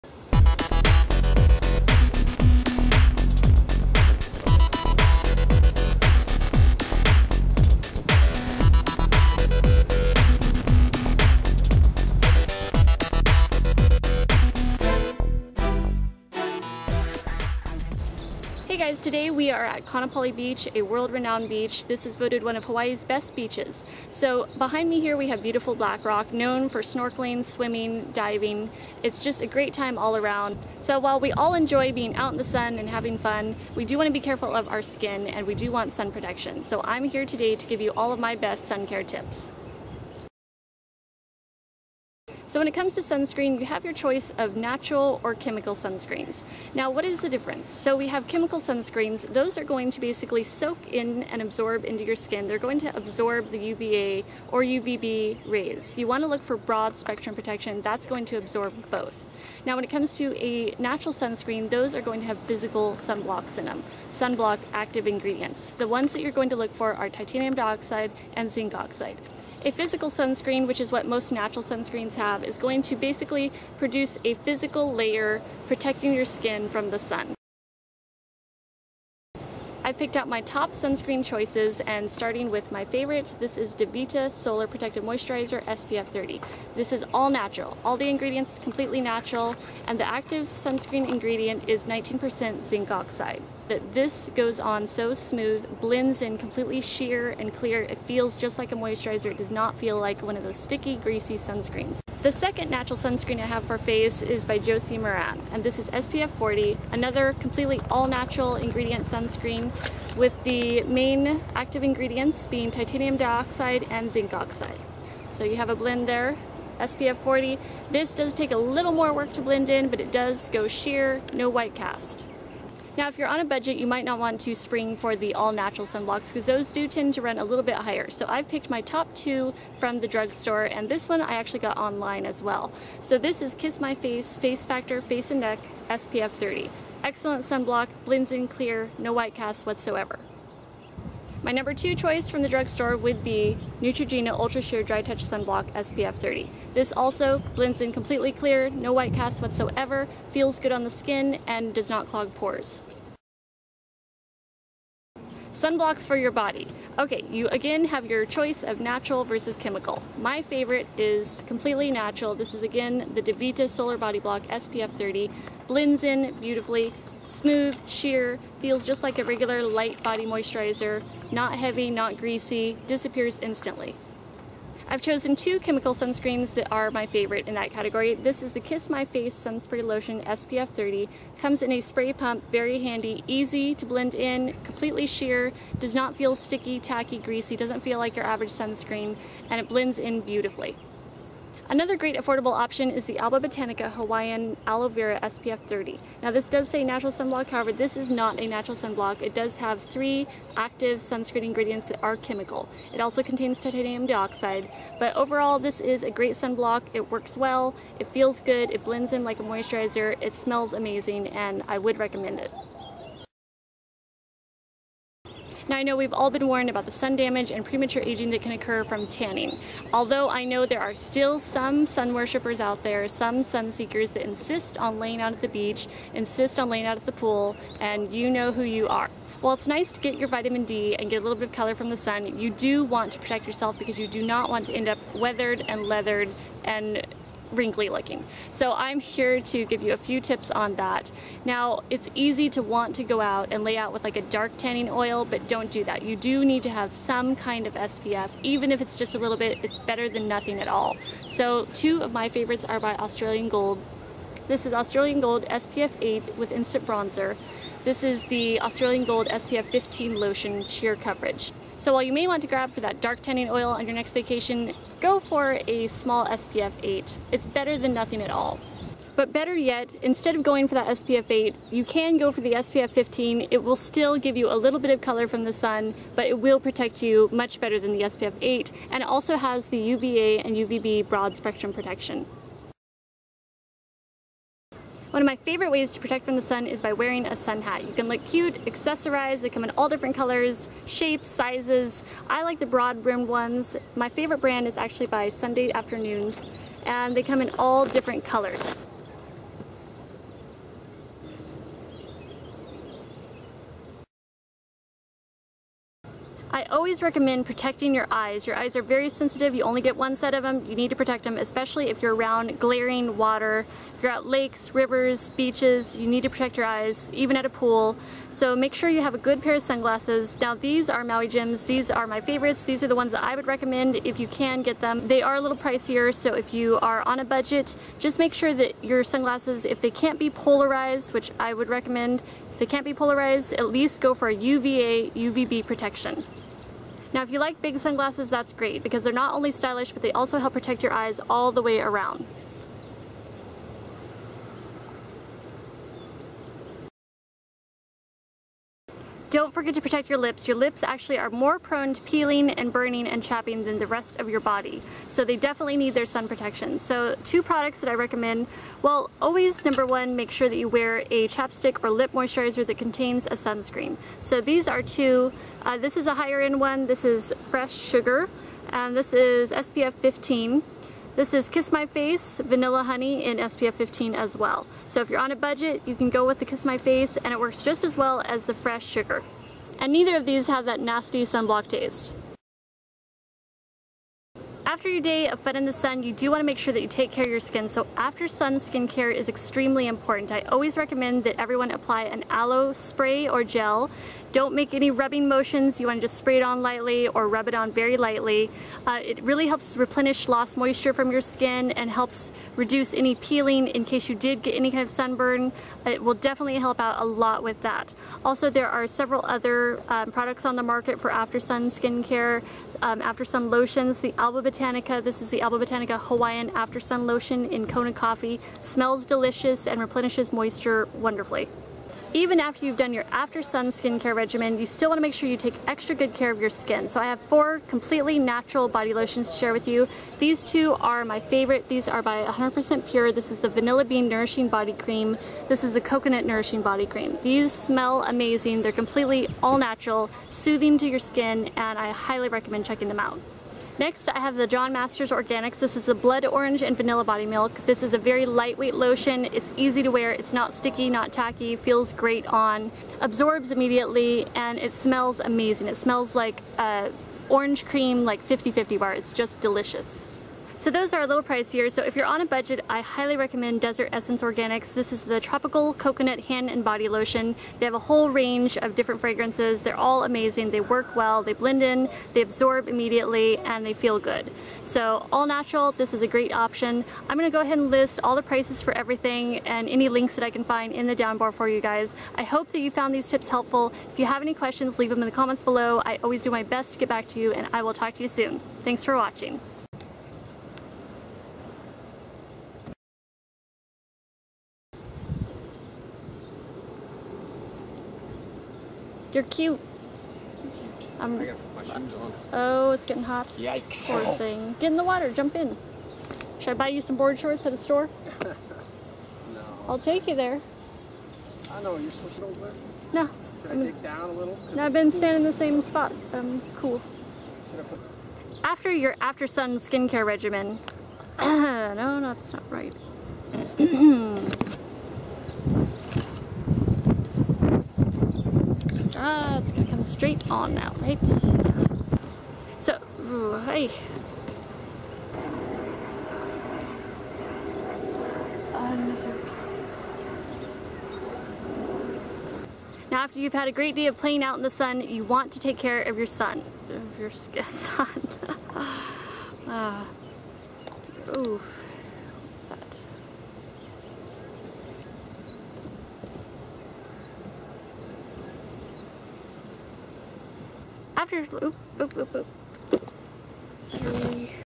Sun Care & Sunscreen Favorites
Aloha beautiful people! These are my favorite products for summer sun care. I hope you find my overview helpful!
***EXPAND FOR INFO***
Where I purchase most of my natural sun care products: Vitacost.com

On my nails: OPI Bubble Bath

Products Mentioned:
FACE:
Devita Solar Protective Moisturizer SPF 30  ($14 @ Vitacost)
*Ideal for daily wear but Not Water Resistant*

Josie Maran Argan Daily Moisturizer SPF 40  ($32 @ Sephora)
Kiss My Face Face Factor SPF 30  ($8 @ Vitacost)
Neutrogena Ultra Sheer Dry Touch Sunblock SPF 30  ($8 @ Walmart)

BODY:
Devita Solar Body Block SPF 30  ($17 @ Vitacost)

Kiss My Face Sun Spray Lotion SPF 30  ($10 @ Vitacost)
Alba Botanica Hawaiian Aloe Vera Sunblock SPF 30  ($6 @ Vitacost)

BEST TANNING: AUSTRALIAN GOLD
Australian Gold SPF 8 Spray Gel w/ Instant Bronzer ($9 @ Walmart)
Australian Gold SPF 15 Sheer Coverage Lotion ($9 @ Walmart)

SUN HATS:
Sun Day Afternoons Sun Hats ($30)

BEST SUNGLASSES:
Polarized - Maui Jims

LIPS:
Fresh Sugar Lip Treatment SPF 15  ($22 @ Sephora)
Kiss My Face Lip Moisturizer SPF 15 ($2 @ Vitacost)
*Or Alba Botanica Very Emollient Sunblock Lipcare SPF 25 ($2 @ Vitacost)

AFTER SUN:
Aloe Spray or Gel ($4 @ Vitacost)
Alba Botanica Hawaiian After Sun Lotion Kona Coffee ($6 Vitacost)

BEST NATURAL BODY LOTIONS:
100% Pure Vanilla Bean Nourishing Body Cream ($15 @ Ulta)
100% Pure Coconut Nourishing Body Cream ($15 @ Ulta)
John Masters Organics Blood Orange & Vanilla Body Milk ($16 Whole Foods)
Desert Essence Organics Hand & Body Lotion ($6 @ Vitacost)